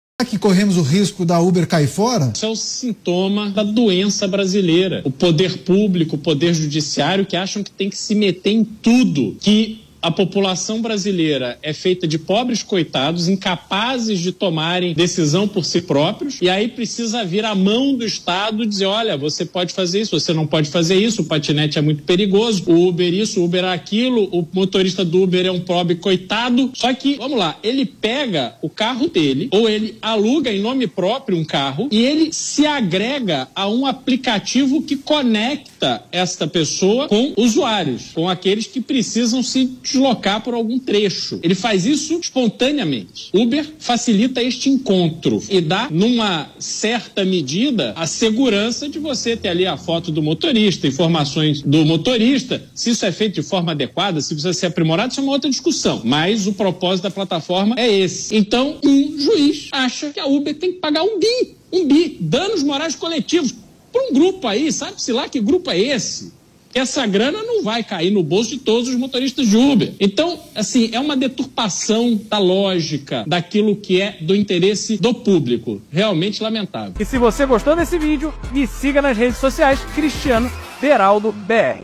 UBER Irá sair do Brasil? Como poderá ficar a situação do aplicativo UBER e os Motoristas?
UBER Irá sair do Brasil? Como poderá ficar a situação do aplicativo UBE